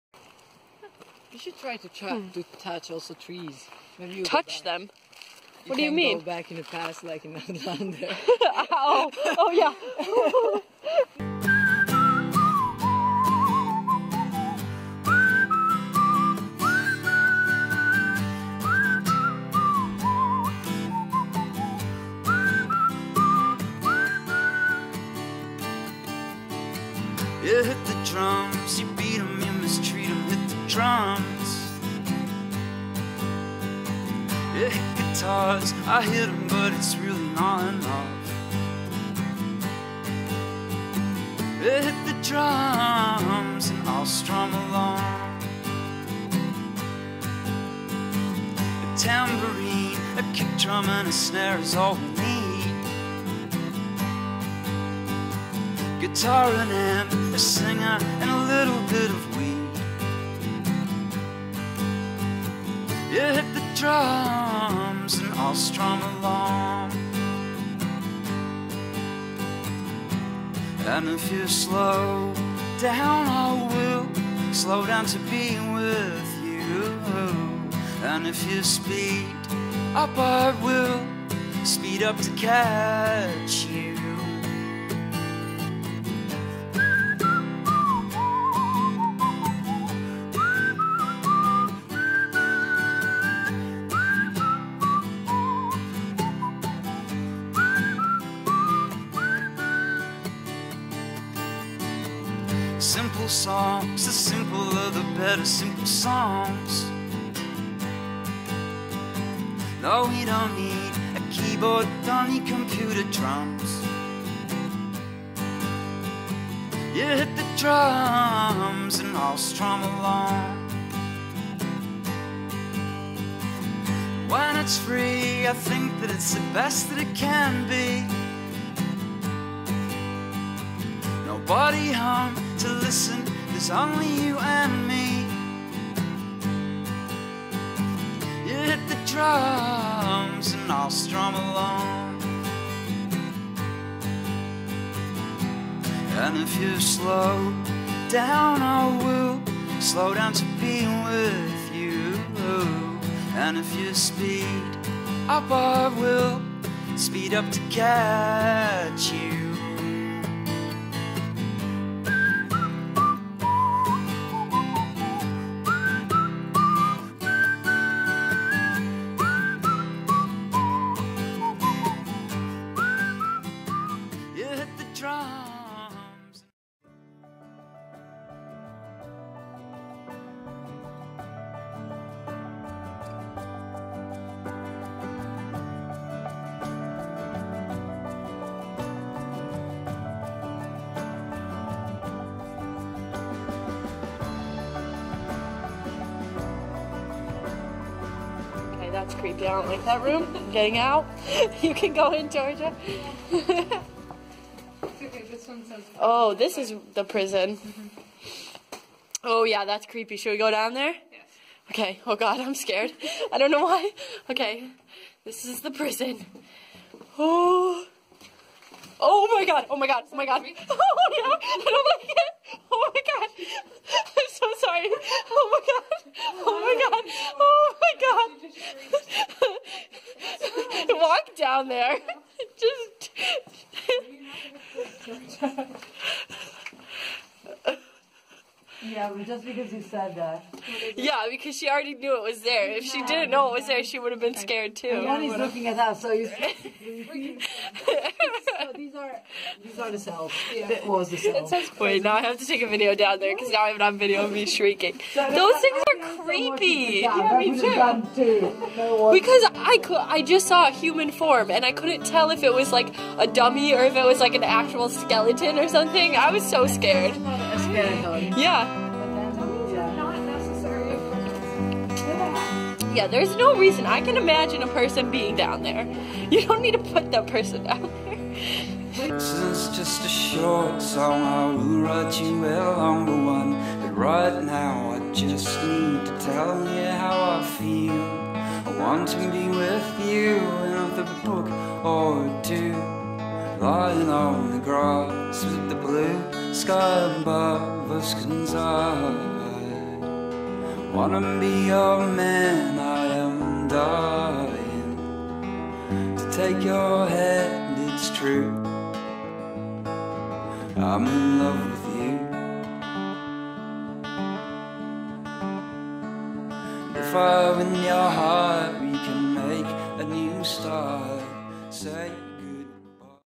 Daytrip to Huntly Castle // GRAD SCHOOL ABROAD
We went to the ruins of a castle and it was pretty cool. Not as cool as Dunnottar though.